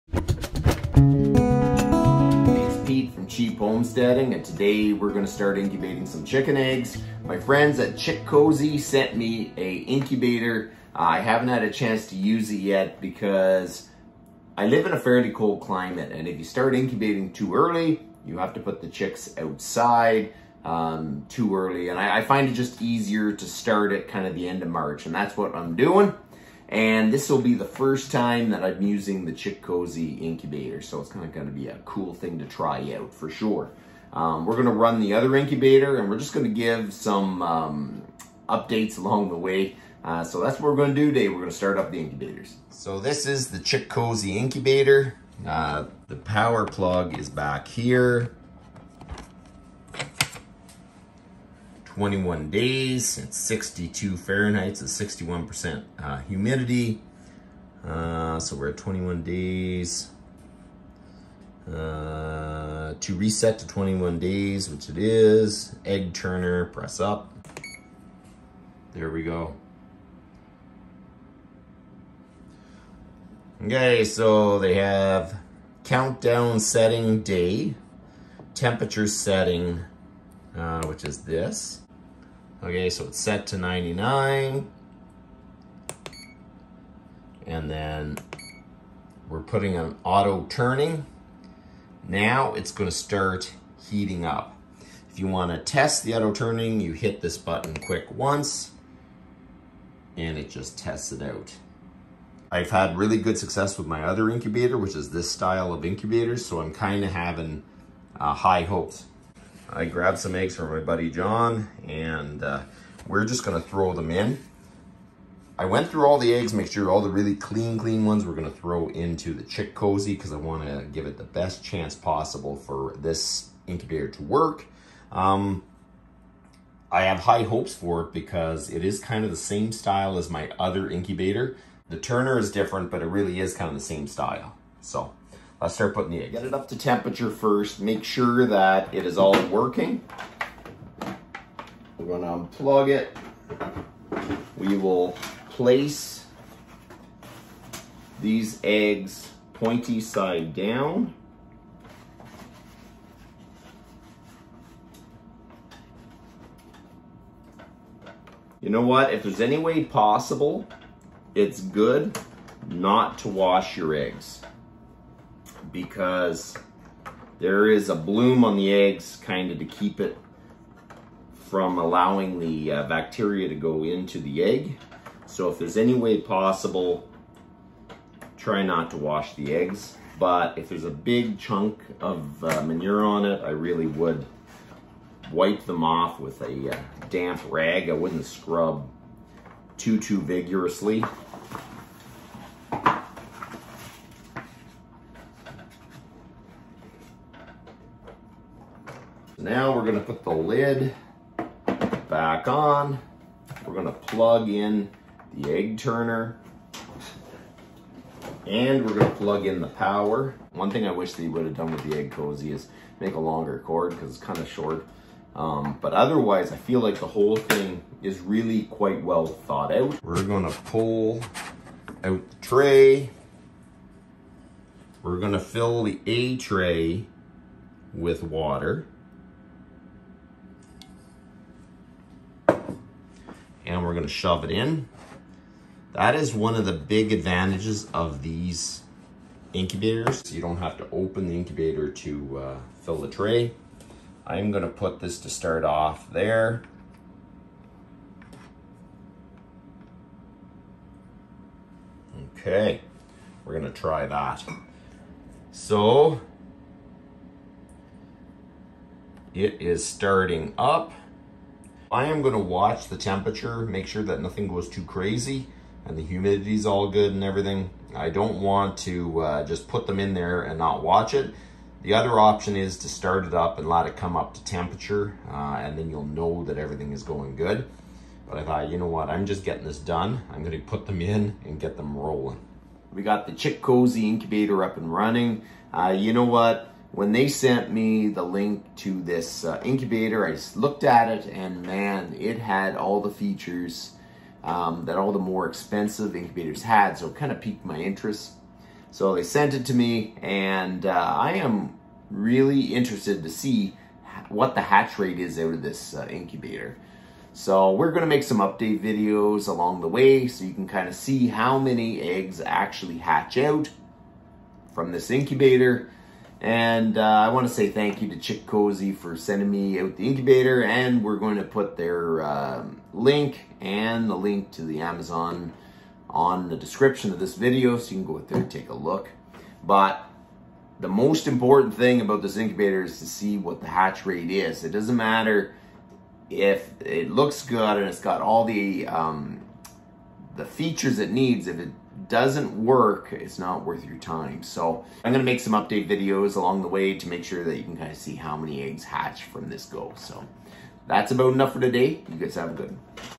Chickcozy Incubator ///How To Hatch Chicks At Home ///￼
Cheap Homesteading is starting up his new egg incubator from Chick cozy. It holds 25 eggs and the display is easy to navigate. The incubator has a lot of good features like automatic egg turners, built in candler. Everything you need to successfully hatch out chicken eggs, or any other eggs you might want to hatch from home.